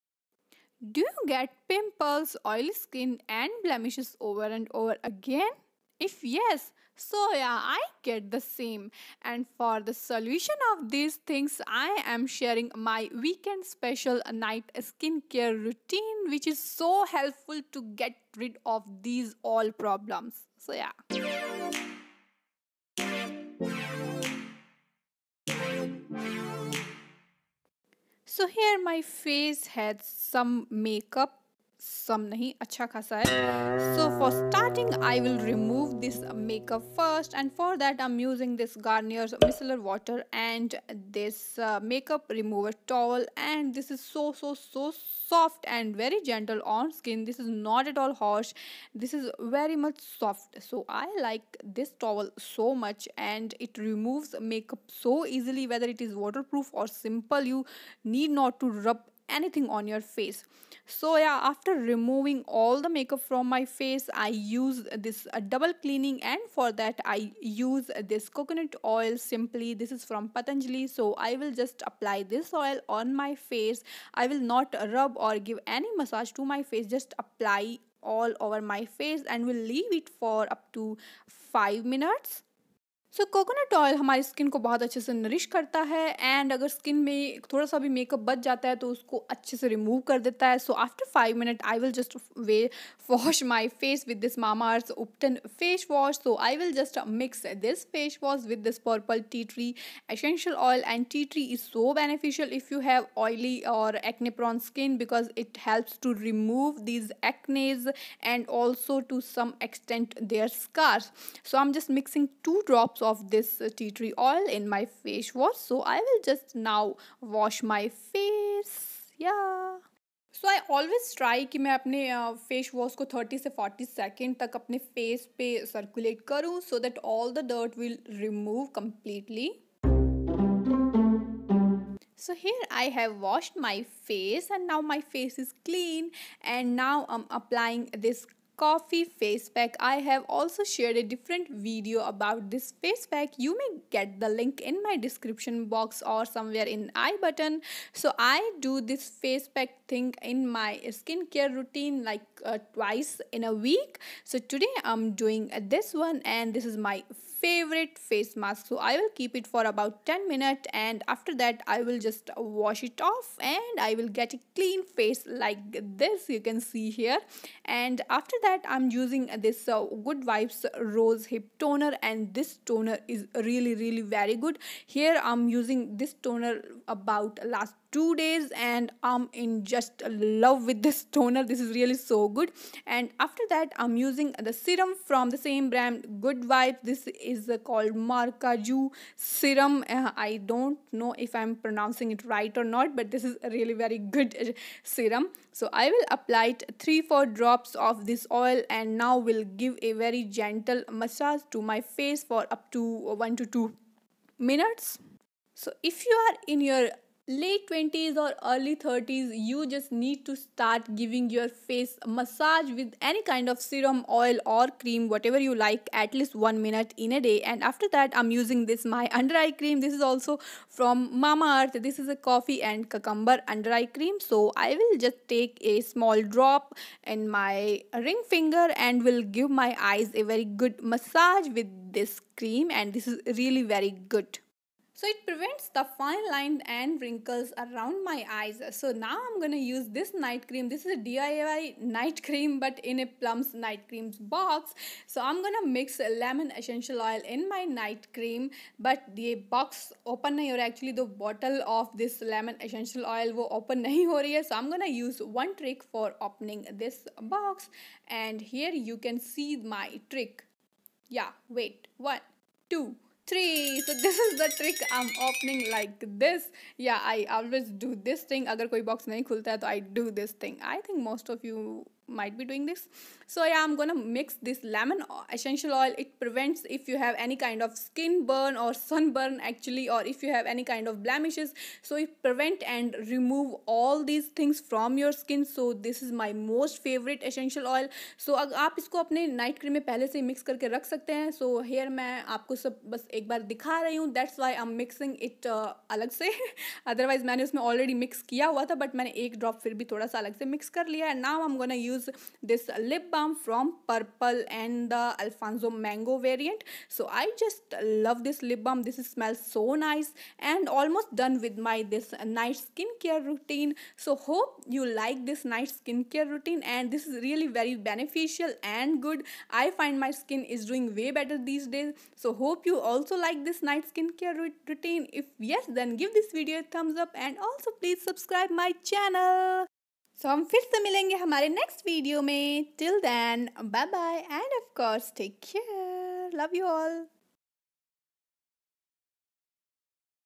How I protect my skin from acne & other skin problems(weekend special)
Weekend special Night skin-care routine for oily n acne prone skin

Hi Dazzlers...

Today I am sharing this for you all..

So,if you like this video, give a thumbs up👍